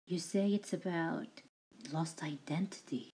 You say it's about... lost identity (w/ Audio)
'Cause iPads from 2016 suck with sound, I guess.

Done today, took me an hour. Just a little practice to try and animate the hair moving slowly and keeping the head on model like the initial key poses.

Animation: Digicel FlipBook
Audio: FlipaClip